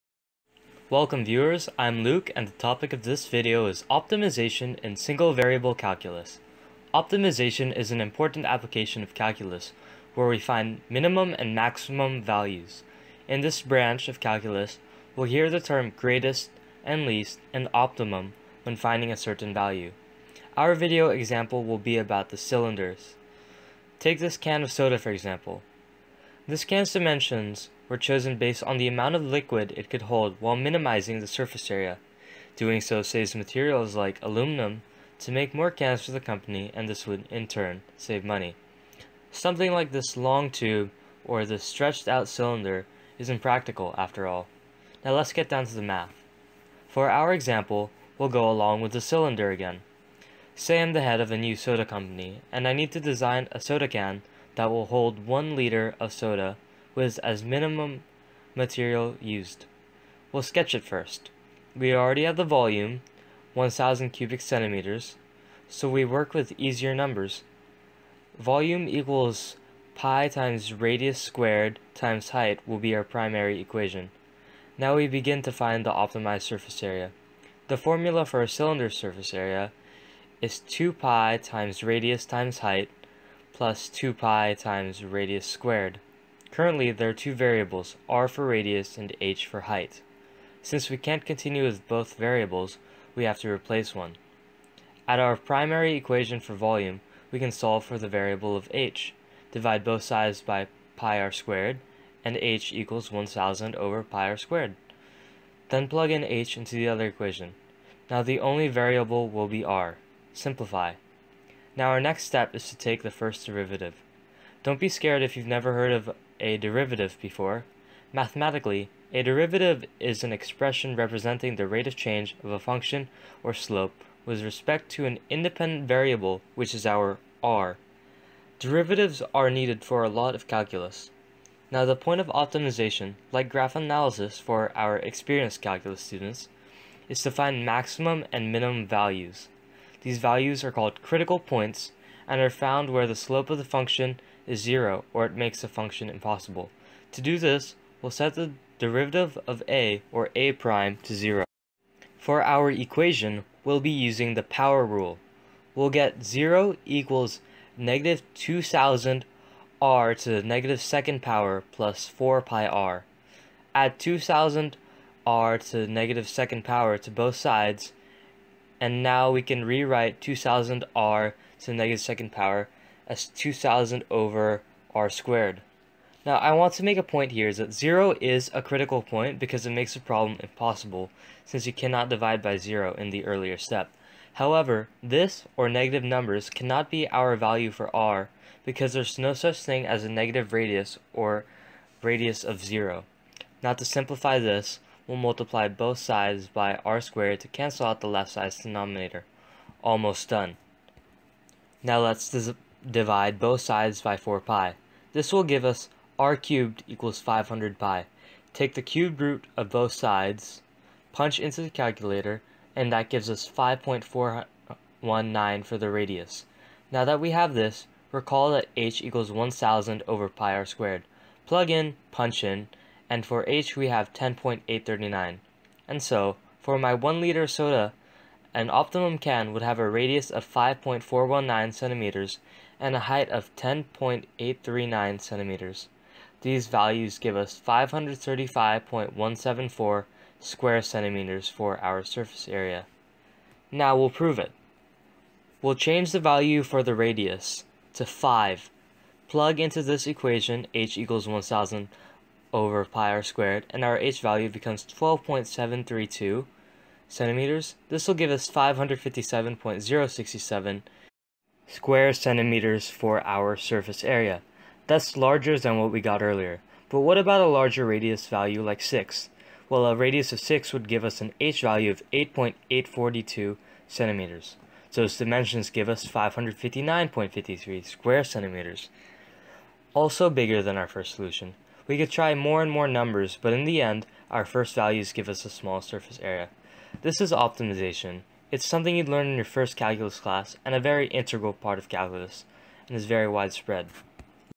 Optimization in Single Variable Calculus
My entry in 2016's Breakthrough Challenge. It's like a "how-to" on optimization using a cylinder as an example.